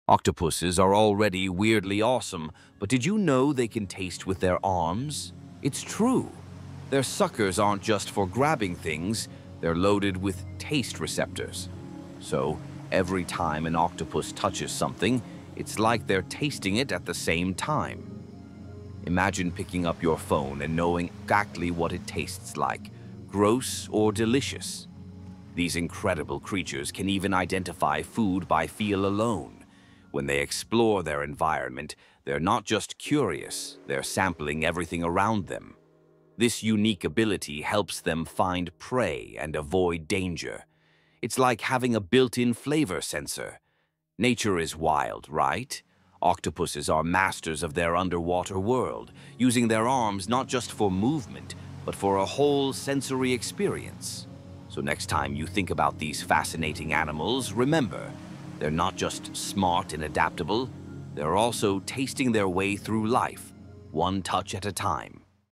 Octopuses Taste with Their Arms?! 🐙😲 Nature is Wild!
Octopuses can taste whatever they touch with their arms — their suckers have taste buds! 🐙🤯 Would you want that superpower?